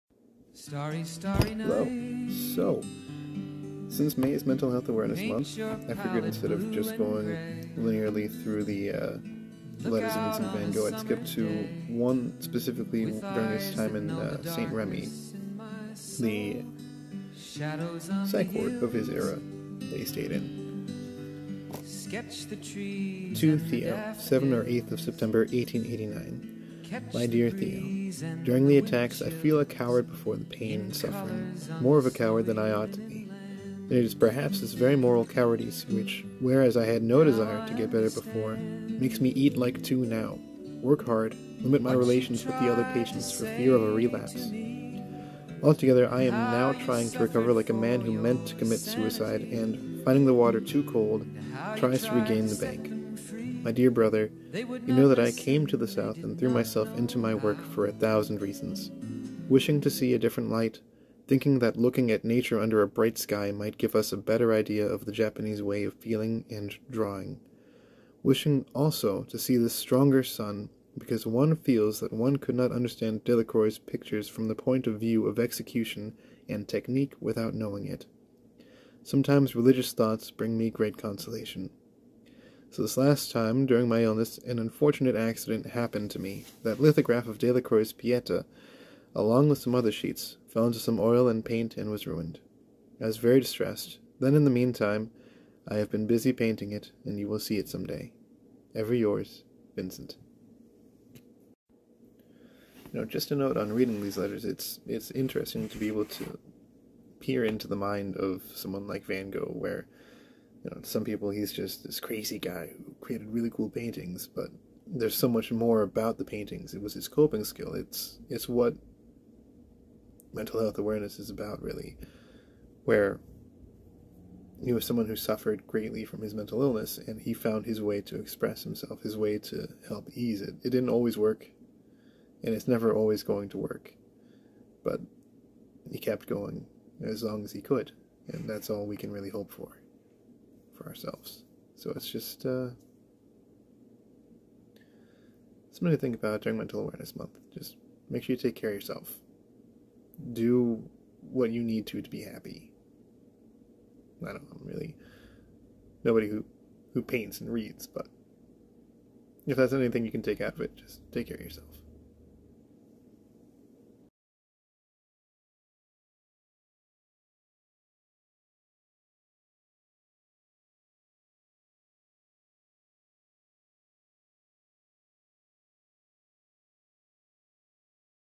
Letters from Vincent pt. 4 mental health awareness month
mental health is important to take care of, not just during Mental Health Awareness month. Talk to a therapist, or a friend to ease your mind. pick up a new hobby to use as a coping skill, get fresh air, do what you need to be happy